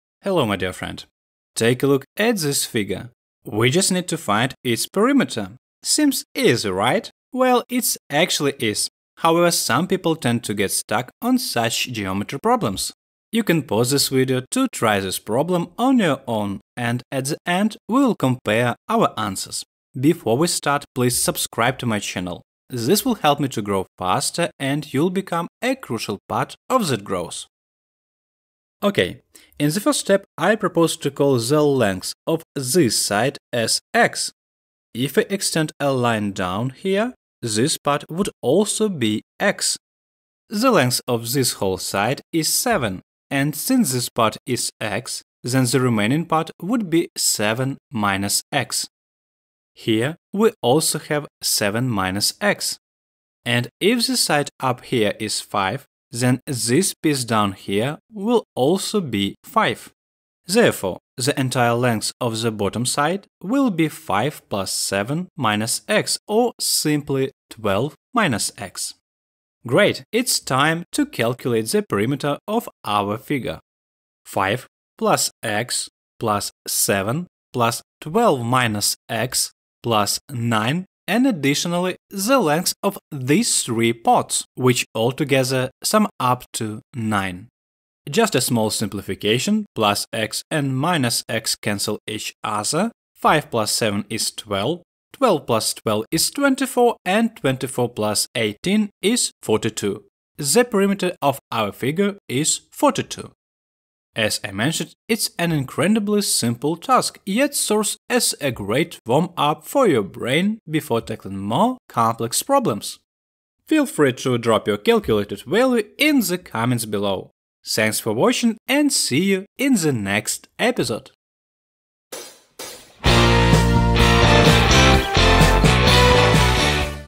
What is the Perimeter of the figure? | Easy Math
Finding the perimeter of these shapes is a piece of cake. Just follow me, and I'll show you how!

Explore more mathematical problems and logic riddles with Math Almanac!